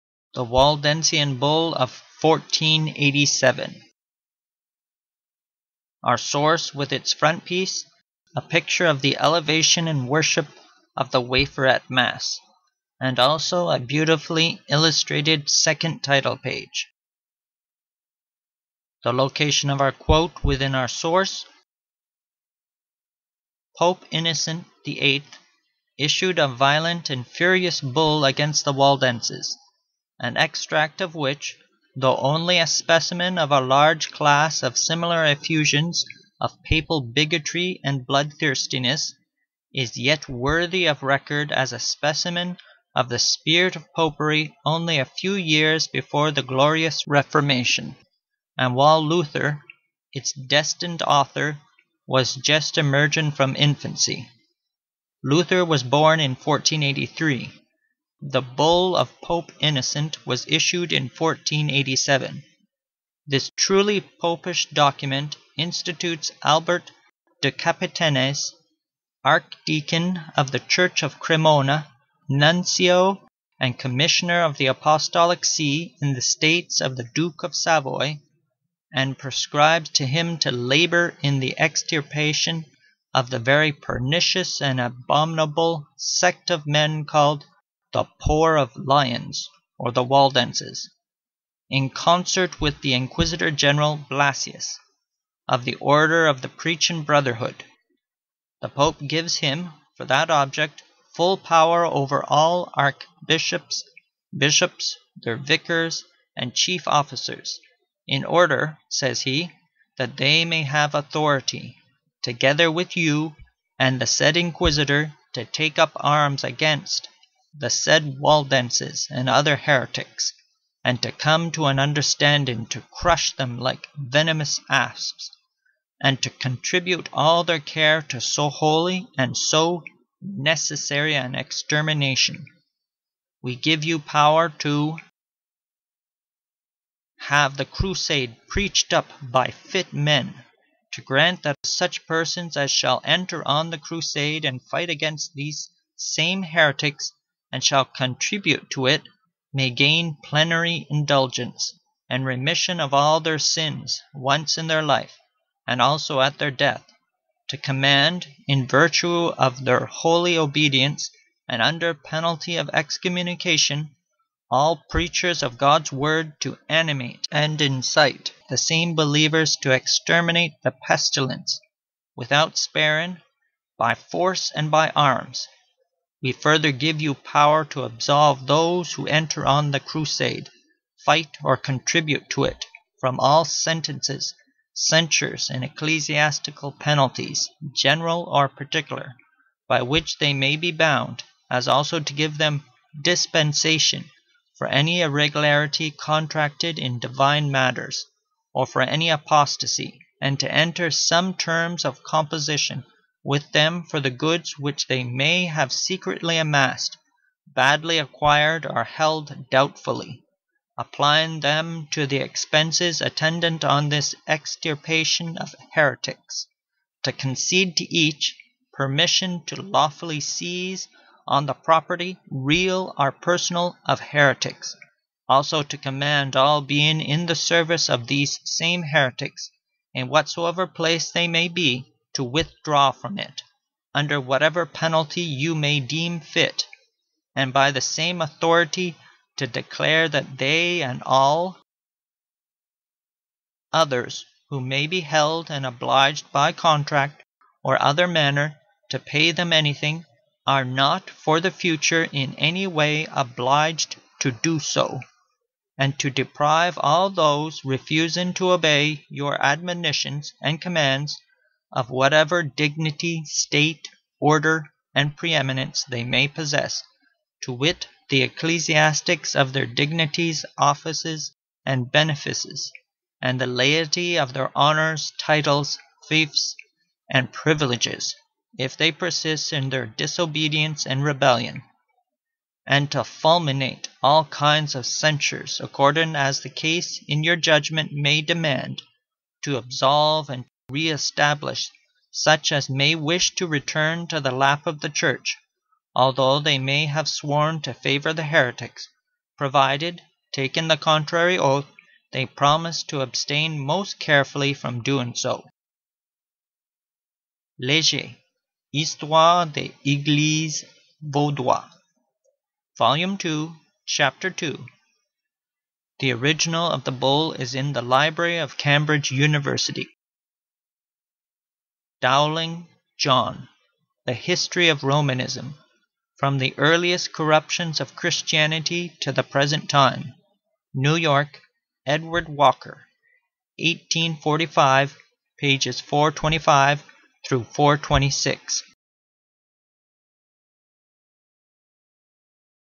The Waldensian Bull of 1487 (Dowling, The History of Romanism, 1845)
An extract from the Papal Bull against the Waldenses, of 1487.